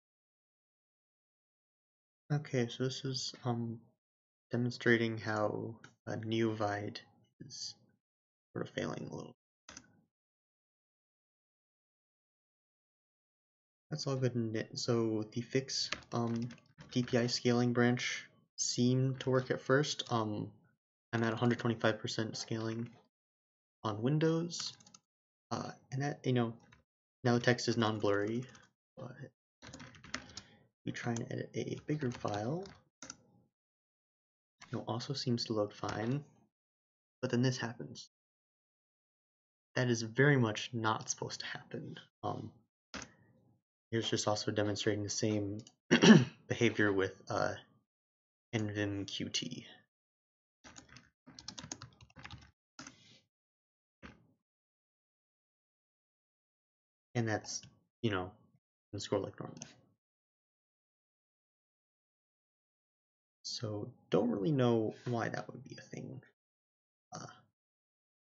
2021-01-03 Neovide fix-dpi-scaling issue
A quick video of how a commit to fix a dpi scaling issue at the time of uploading) turned out to not work.